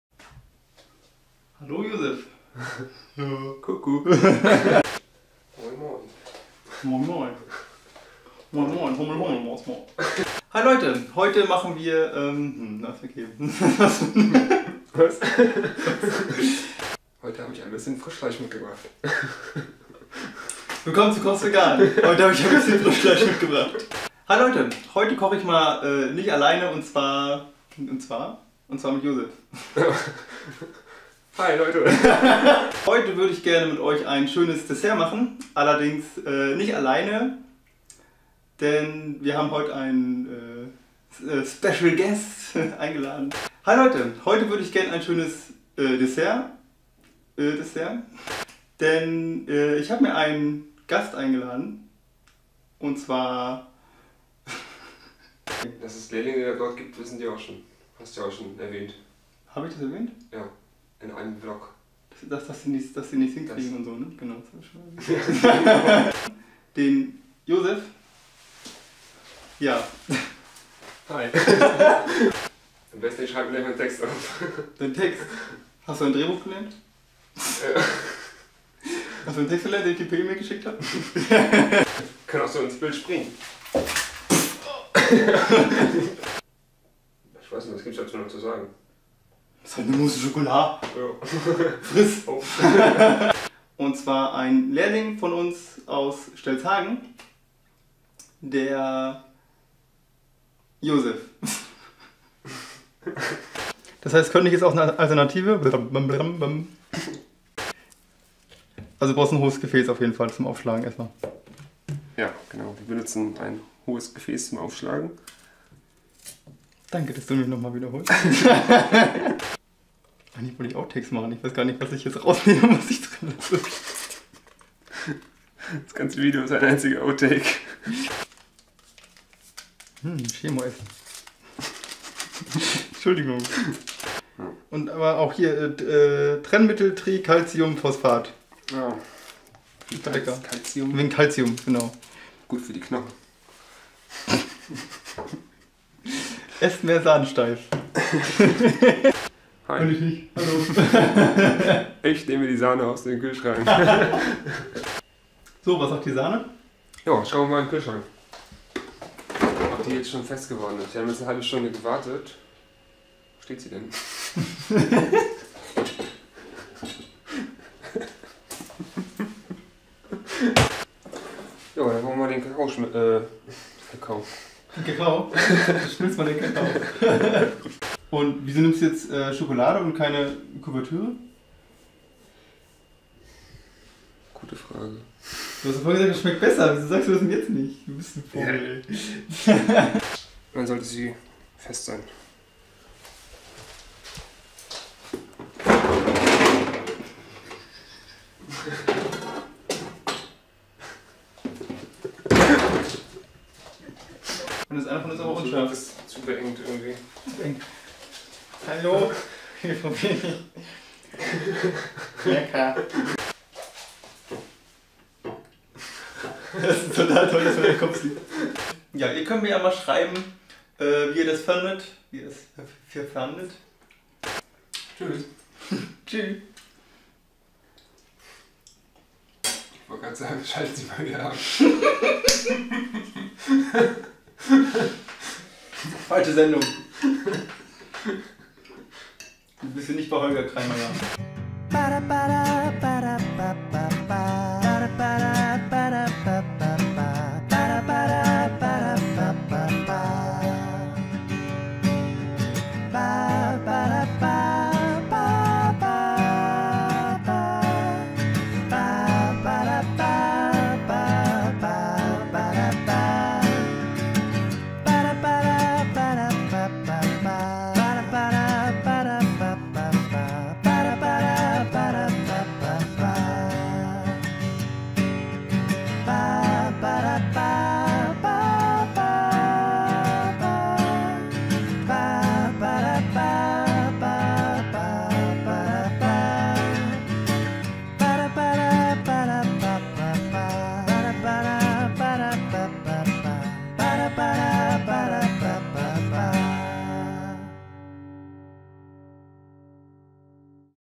[OUTTAKES & PANNEN] Mousse Au Chocolat Selber Machen - Veganes Schokomousse Rezept von Koch's Vegan
Das sind die Patzer, die beim Kochvideo mit Josef passiert sind. ;) Viel Spaß beim Anschauen.

Zutaten: